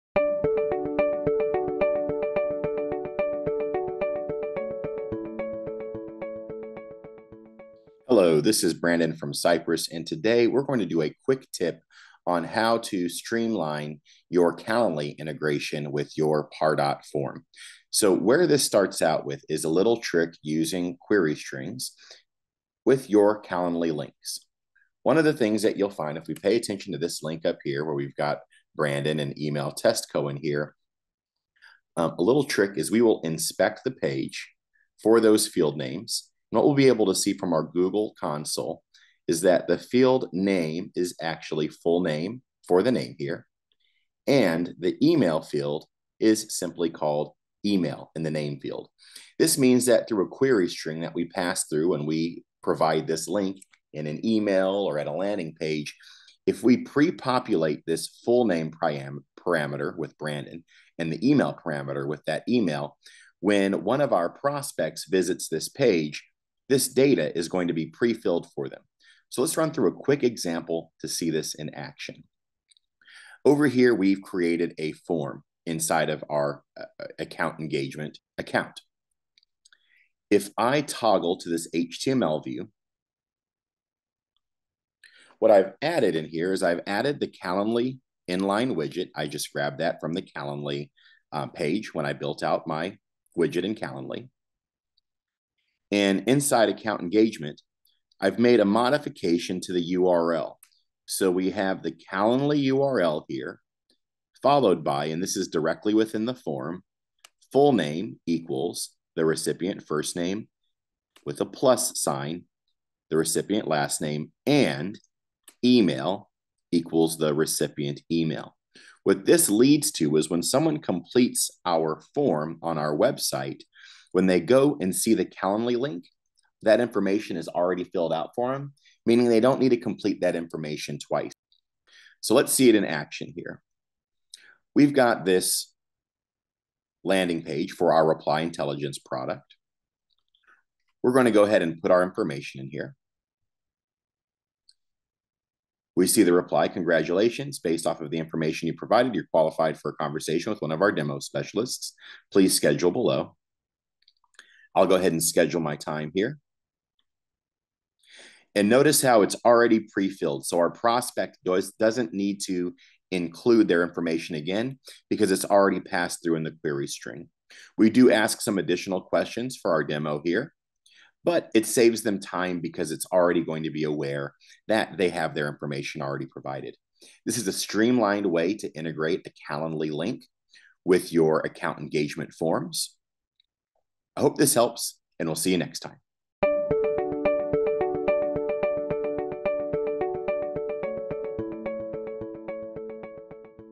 Calendly and Pardot Forms:
Link your Account Engagement Forms (Pardot) to a Calendly registration page with this streamlined solution! This works great for demo requests, info forms.

Reduce schedule abandonment by streamlining your Calendly registration process with Pardot Forms!